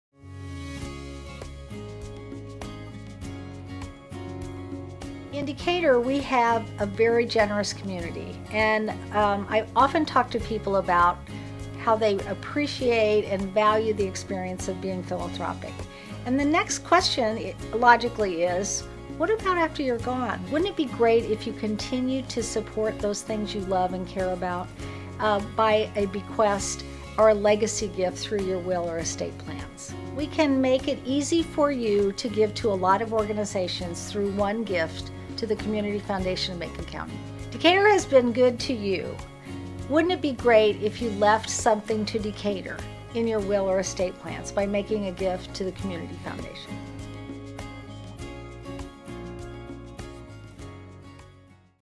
Community Foundation - Legacy
Wegi Stewart talks about how you can leave a legacy to the non-profits you care about with the Community Foundation of Macon County.